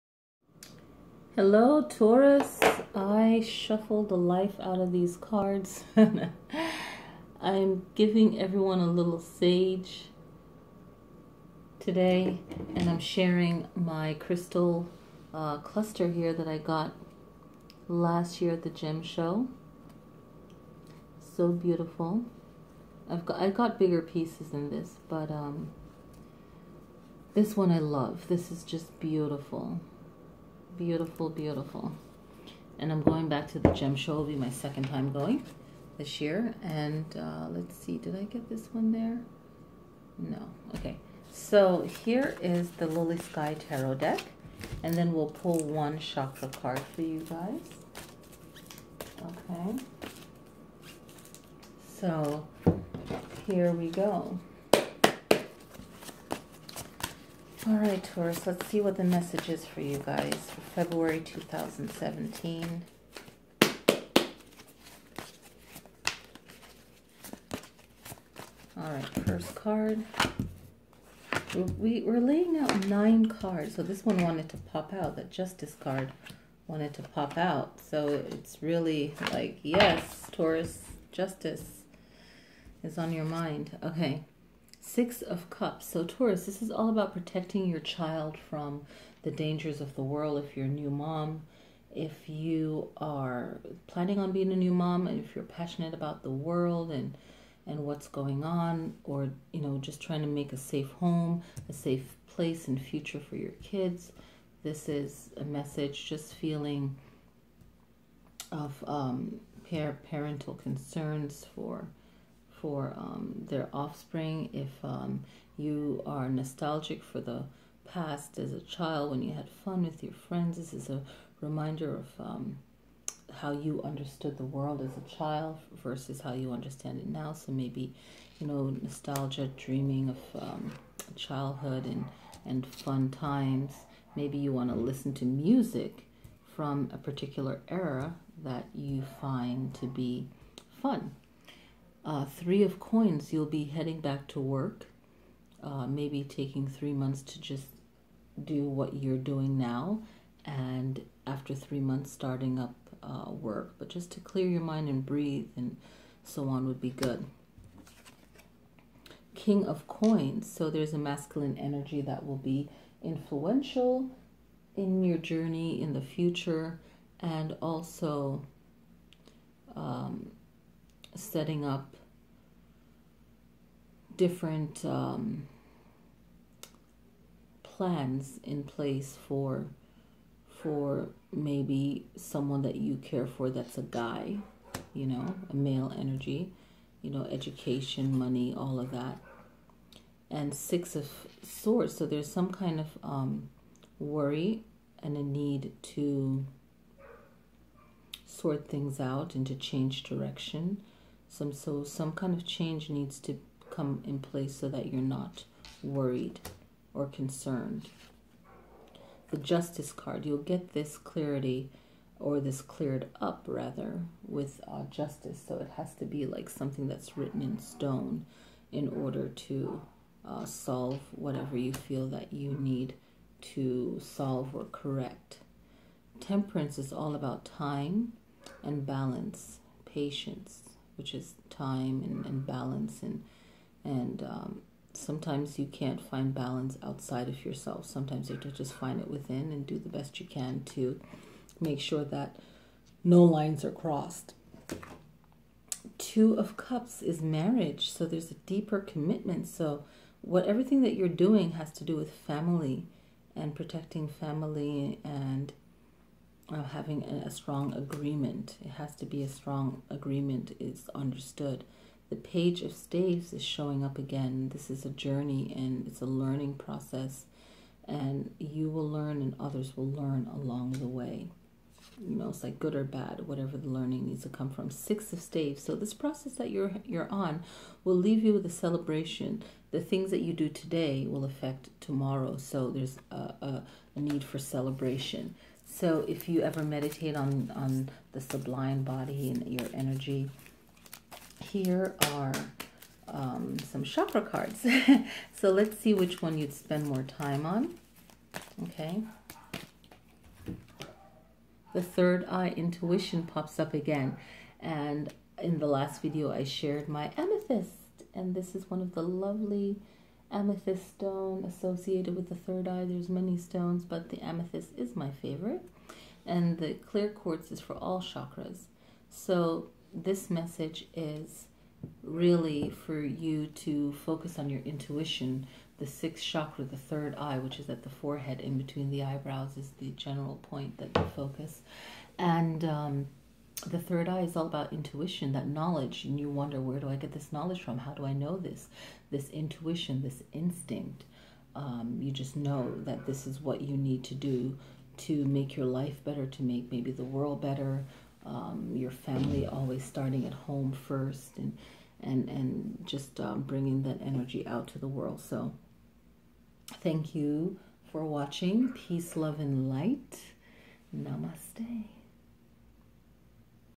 Taurus February 2017 General Tarot Reading
Taurus February  2017~General Tarot Card Reading

LIKE FACEBOOK: Lilly Sky Tarot

Cards Used: Lilly Sky Tarot Deck

FACEBOOK: Lilly Sky Tarot   (Message me)
Periscope: LillySkyTarot